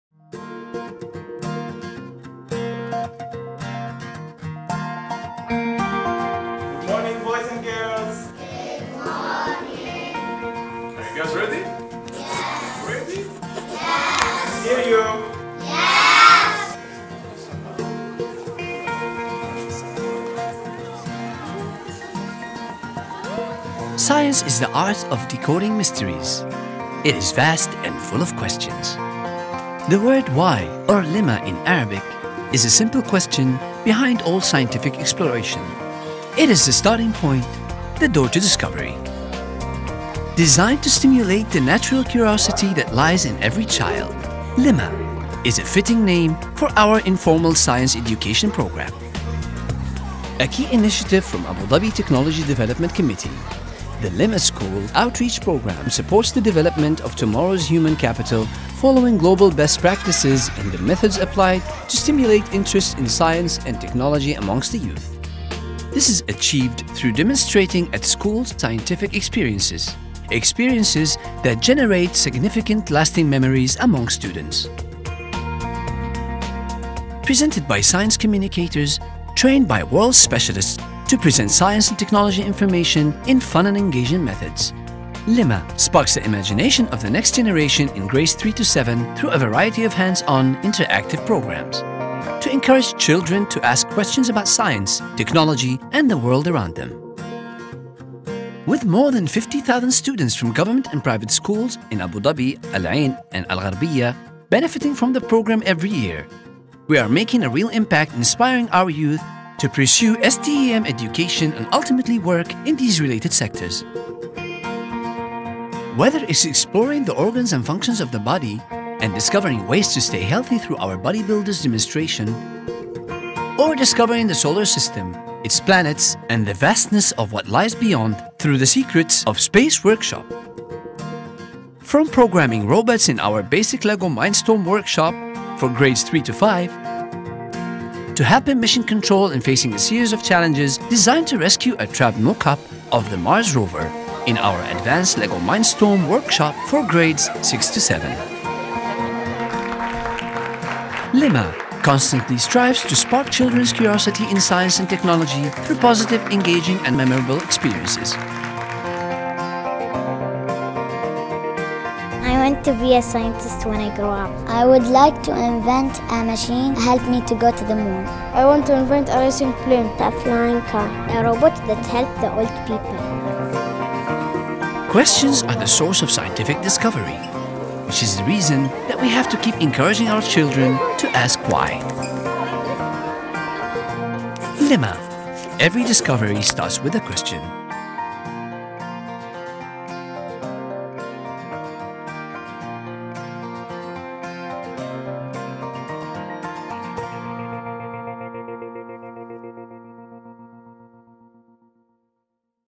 Lema: Informal science program for schools
Mubadala and the Abu Dhabi Technology Development Committee (TDC) have partnered to launch “lema?”, an exciting new learning initiative.

The “lema?” schools program is an informal science education initiative that aims to raise awareness about scientific exploration through a series of engaging workshops, inspiring the youth (grades 3 – 7) in the fields of science and technology.